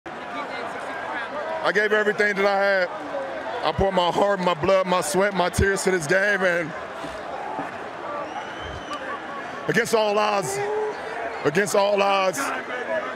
LeBron James saying "I gave everything that i have!" - NBA meme - Green Screen
Free to use Green Screen Meme Template of LeBron James giving a speech after winning the 2016 NBA Finals (Cleveland Cavaliers Vs Golden State Warriors)

For personal use only. Redistribution is prohibited.